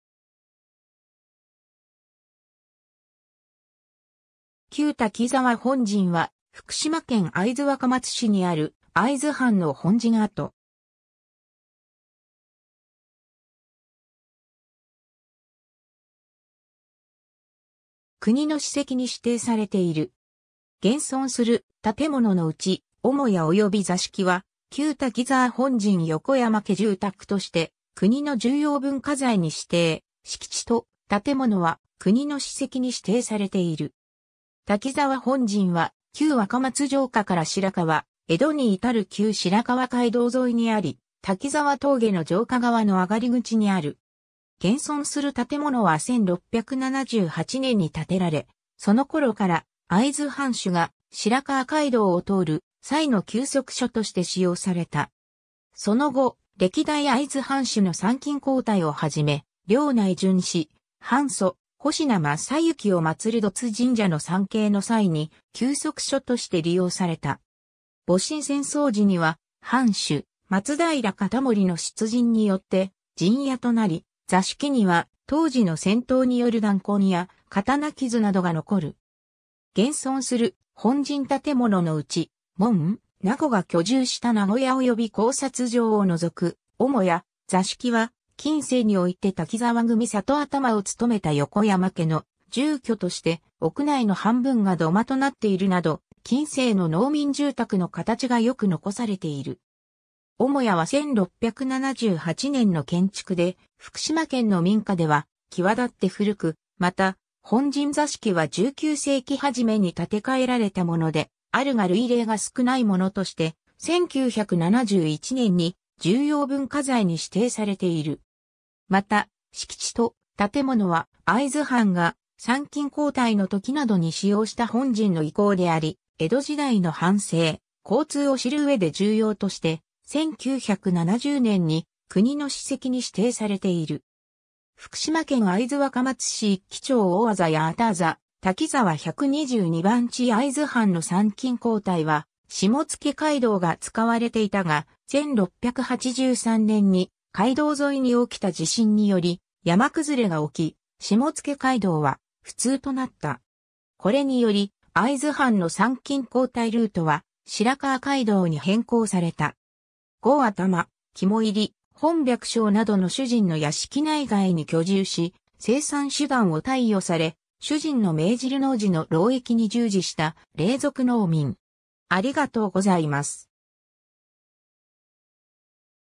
旧滝沢本陣
重要文化財_(建造物)
旧滝沢本陣（きゅうたきざわほんじん）は、福島県会津若松市にある会津藩の本陣跡。
現存する建物のうち、主屋および座敷は、旧滝沢本陣横山家住宅（主屋・座敷）として国の重要文化財に指定、敷地と建物は国の史跡に指定されている。
 滝沢本陣は、旧若松城下から白河、江戸に至る旧白河街道沿いにあり、滝沢峠の城下側の上り口にある。
現存する建物は1678年（延宝6年）に建てられ、その頃から、会津藩主が白河街道を通る際の休息所として使用された。
その後、歴代会津藩主の参勤交代をはじめ、領内巡視、藩祖保科正之をまつる土津神社の参詣の際に休息所として利用された。
戊辰戦争時には、藩主松平容保の出陣によって陣屋となり、座敷には当時の戦闘による弾痕や刀傷などが残る。
 　 現存する本陣建物のうち、門、名子が居住した名子屋および高札場を除く主屋・座敷は、近世において滝沢組郷頭を勤めた横山家の住居として、屋内の半分が土間となっているなど、近世の農民住宅の形がよく残されている。
主屋は1678年の建築で、福島県の民家ではきわだって古く、また、本陣座敷は19世紀初め（文化文政期）に建て替えられたものであるが類例が少ないものとして、1971年（昭和46年）に重要文化財に指定されている。
 また、敷地と建物は、会津藩が参勤交代のときなどに使用した本陣の遺構であり、江戸時代の藩政、交通を知るうえで重要として、1970年（昭和45年）に国の史跡に指定されている。
 福島県会津若松市一箕町大字八幡字滝沢122番地 ^ 会津藩の参勤交代は下野街道が使われていたが、1683年（天和3年）に街道沿いにおきた地震により山崩れがおき、下野街道は不通となった。
これにより、会津藩の参勤交代ルートは、白河街道に変更された。
 ^ （なご） 郷頭、肝煎、本百姓などの主人の屋敷内外に居住し、生産手段を貸与され、主人の命じる農事の労役に従事した隷属農民。